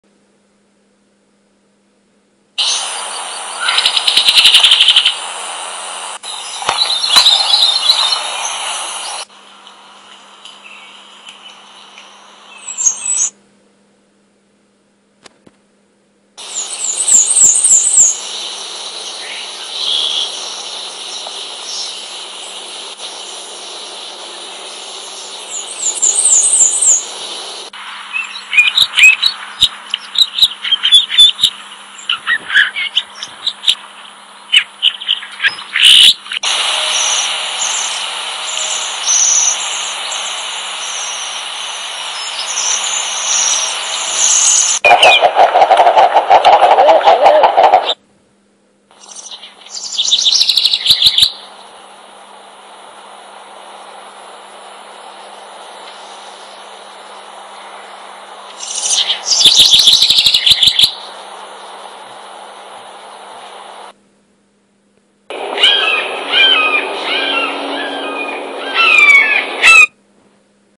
North American Birds Sounds Android App
Presenting a North American Birds Sounds compilation app with high quality sounds and songs of birds found in North America!